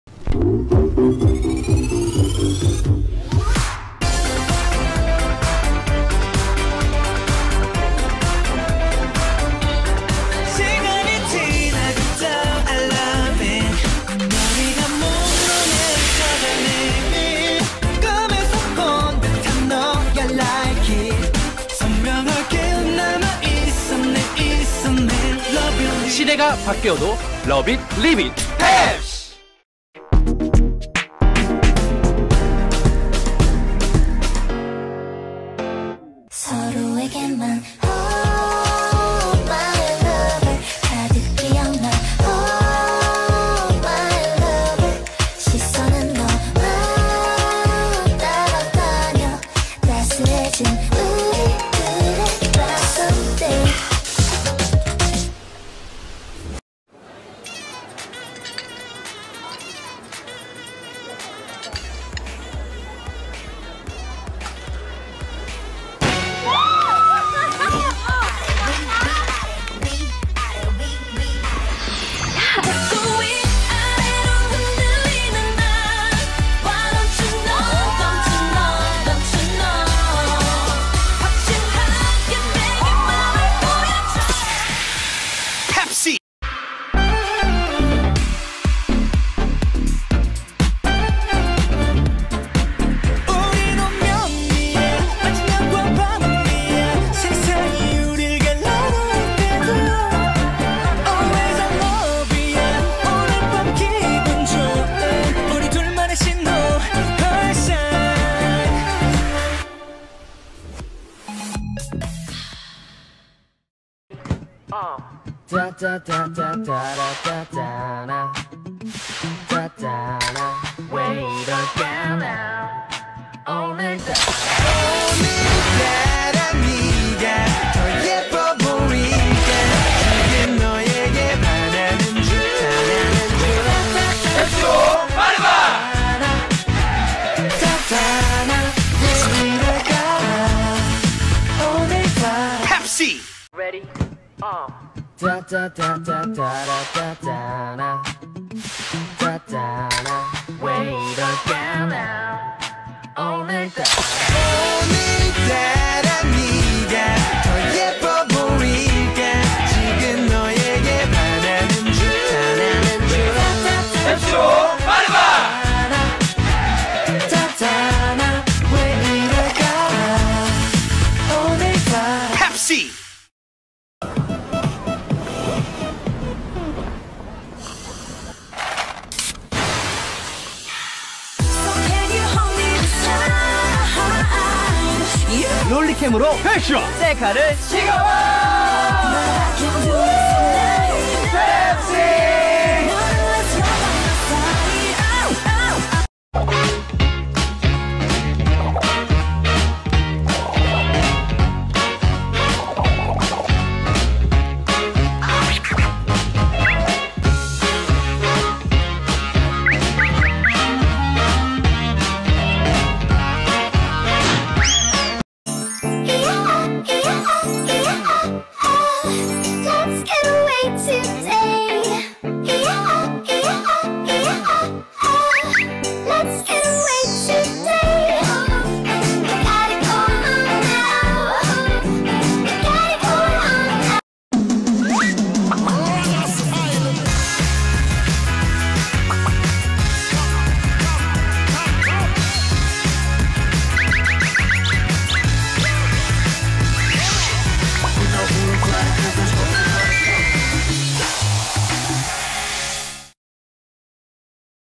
Pepsi Commercials by K-pop Celebrities
YDPP 와이디피피, EUNHA 정은비 (GFriend), RAVI (VIXX), EXID, Ong Seong Wu 옹성우 (Wanna One), Wanna One and iKon.